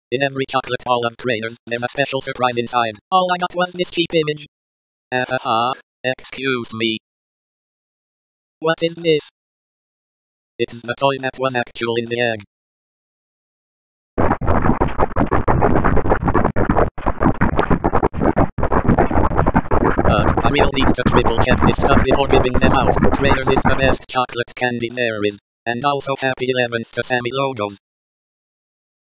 Dano Craners - Craner the Fox Toy (2021, USA)
Introducing, Craner the Fox, the Craners chocolate balls mascot. There's always a special surprise inside each chocolate ball waiting for you. But beware, sometimes, the prize isn't as expected!

Also, Happy (Late) 11th Birthday to SammyLogos!